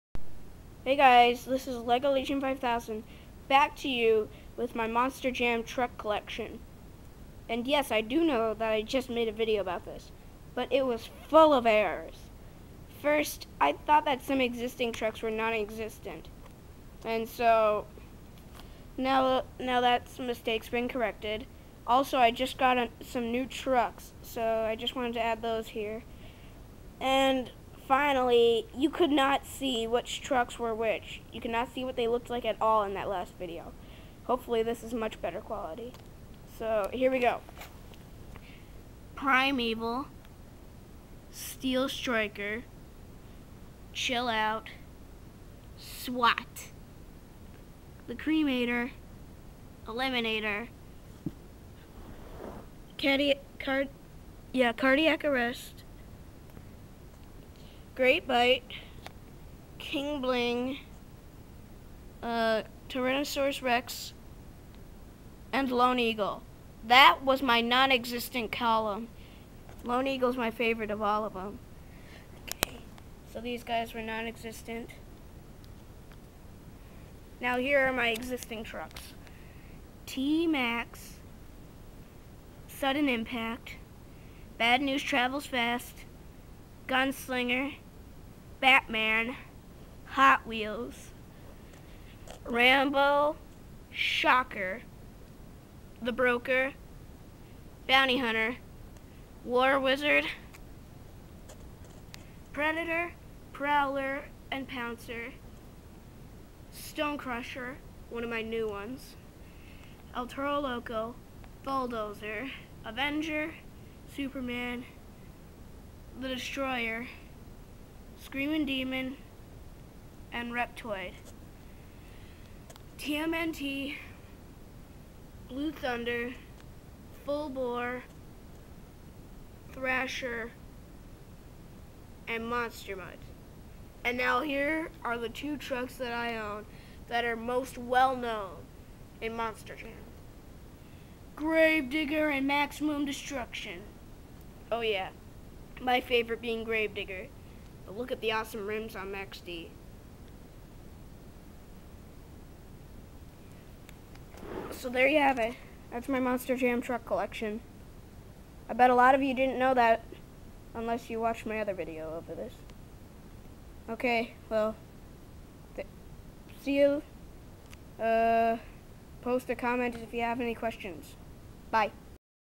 LL5's Monster Truck Collection: November 8, 2009
If you've looked at the 'About Me' section on my channel recently, you'd know I'm in to Monster Jam. This is my collection of toy monster trucks. Hope u enjoy! Dedicated to SS, TWE, OAE, TAC2, Jamesis5, TWR, Thomasfan24, and Supermariofan682.